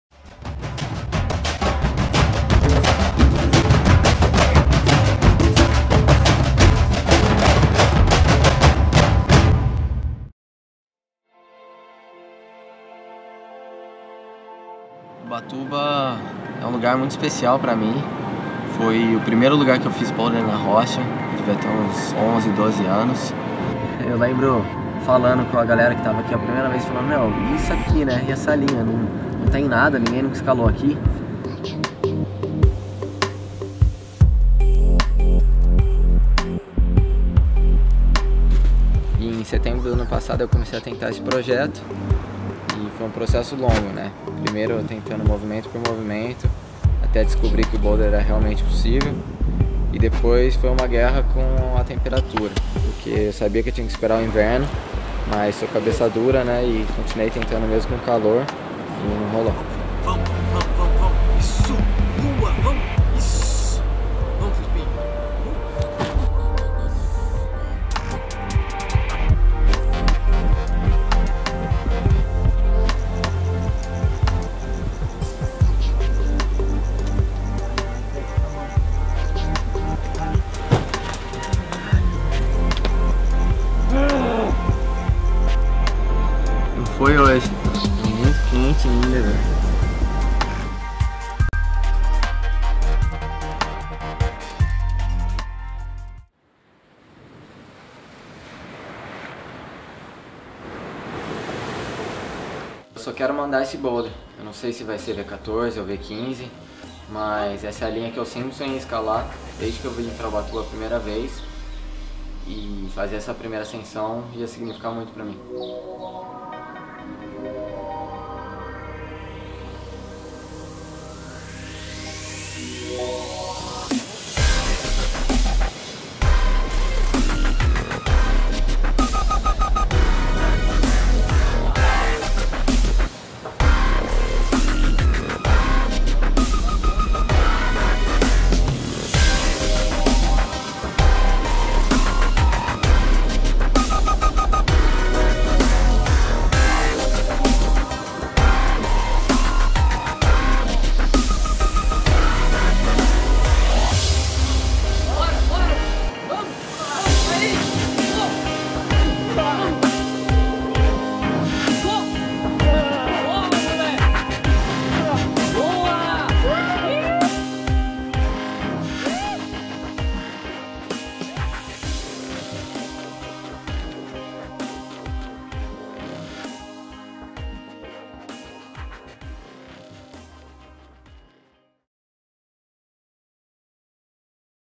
Local Hero Felipe Camargo Establishes Brazil's First V15 Boulder | The Thrill Of Brazil, Ep. 2
Sometimes in climbing the stars just align for you and everything comes good in a single, perfect moment. When Felipe Camargo made the first ascent of 'Fortaleza' (V15/8C) On July 13th 2014, this was one such moment. The line of 'Fortaleza' had stood as a project for over 12 years, its imposing architecture dominating the wave-washed platform of Ubatuba; on which it stands. Felipe had known about the project since his first time climbing outdoors when he actually completed some easier lines on the same boulder. Over time, he returned to Ubatuba and gradually began to piece together the moves on this incredibly difficult project. Despite countless attempts, Felipe was stymied again and again by difficult conditions or his own performance. Until the day of the World Cup final that is, when, with Brazil no longer in the competition, Felipe ignored the football and instead spent the day at Ubatuba where he finally topped out the project. Perhaps we should all be grateful that Brazil was knocked out in the Semis. If not, one of the the country's hardest boulders might still be waiting for its first ascent!

Director: Bruno camargo
Producer: Bruno camargo